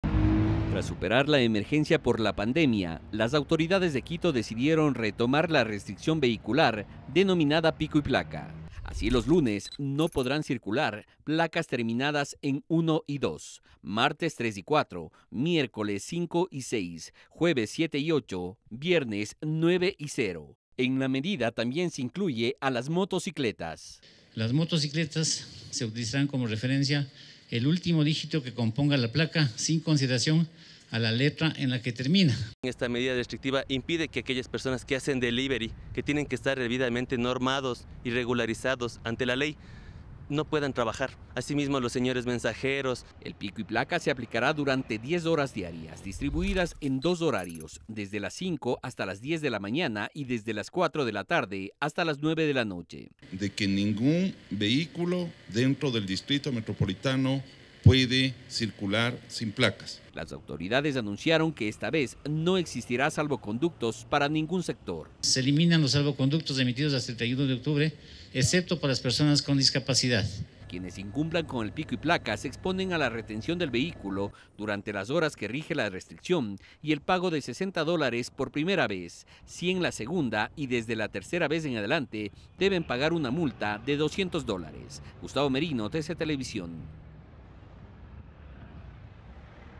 EL PICO Y PLACA ENTRA EN VIGENCIA ESTE 4 DE NOVIEMBRE. SE INCLUYE TAMBIÉN A MOTOCICLETAS.
Desde este  4 de noviembre retorna al distrito metropolitano de Quito la medida de restricción vehicular Pico y Placa. Este jueves por ejemplo no podrán circular durante 10 horas los autos que tengan placa terminada en los dígitos 7 y 8.

JAIME ERAZO - DIRECTOR DE MOVILIDAD DEL MUNICIPIO DE QUITO
DARÍO PALADINES - ASOMOTO
SANTIAGO GUARDERAS - ALCALDE DE QUITO